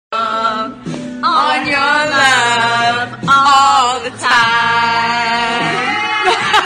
Charlotte flair and Alexa bliss singing 😘😘
check out this, new video on my YouTube channel 😉

one of the most adorable moment of Charlotte and Alexa ❤️ singing together ❤️❤️